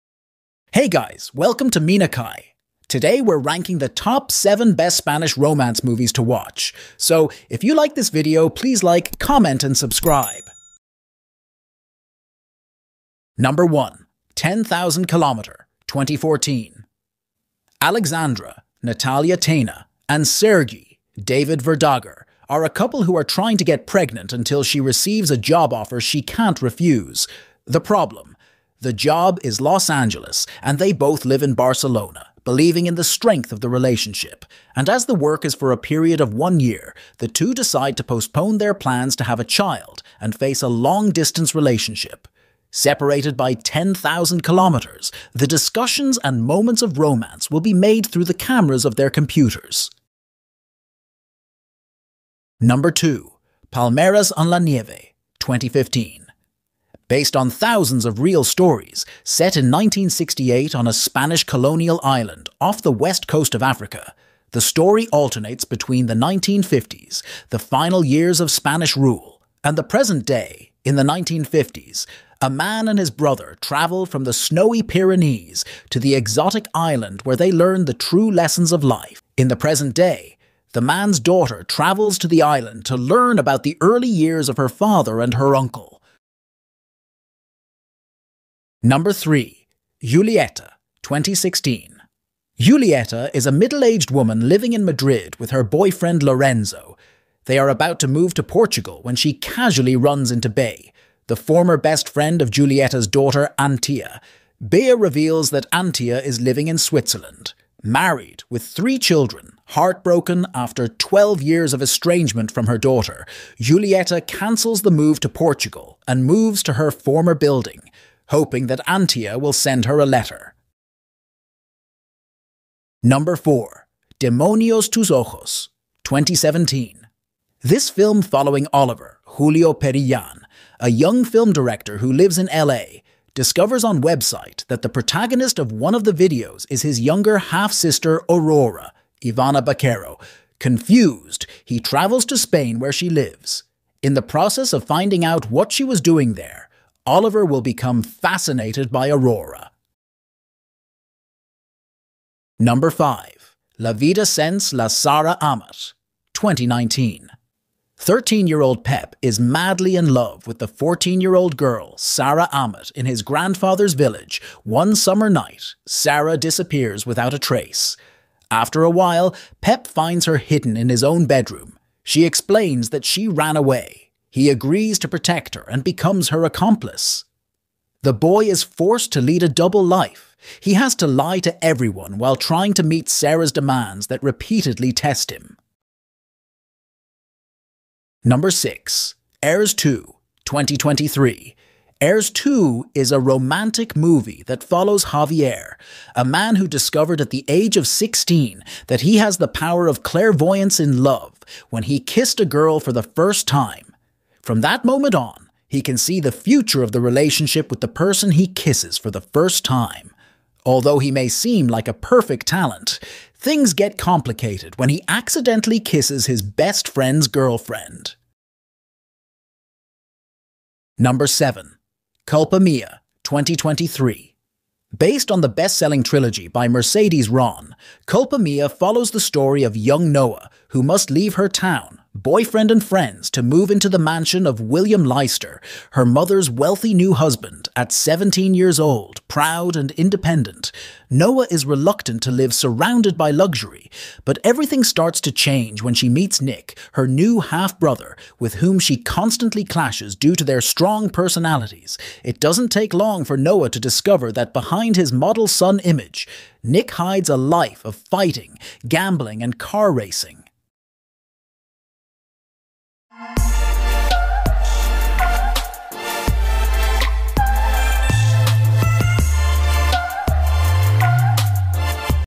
Top 7 Spanish Romance Movies
Hey guys, Welcome to Minakai. Today, we're ranking the top 7 best spanish romance movies to watch.